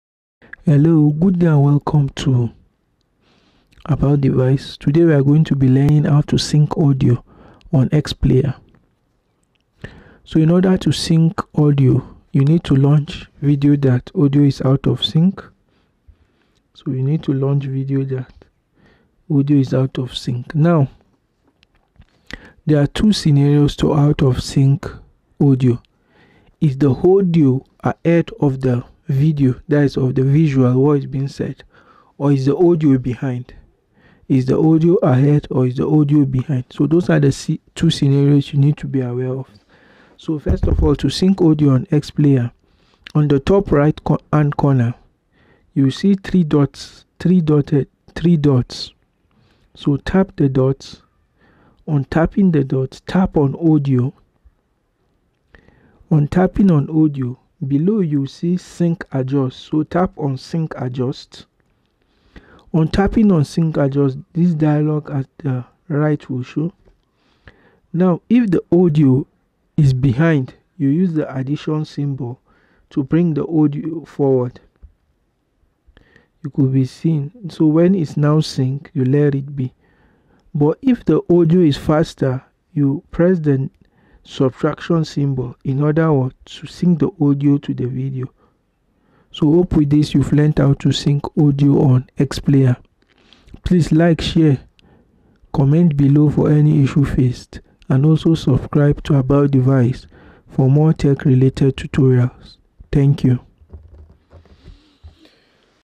Sync Audio on XPlayer Android
How To Sync Audio On Video Player All Format - XPlayer for Android
Adjust Audio Delay in XPlayer (Android)

- (Subtraction) is to be used when audio is ahead of video
+ (Addition) is to be used when audio is behind the video

Adjust Audio Speed
Adjust Audio Delay

Fix Audio delay issues, audio is ahead of Video or Visuals
Fix Audio is behind the Video or Visuals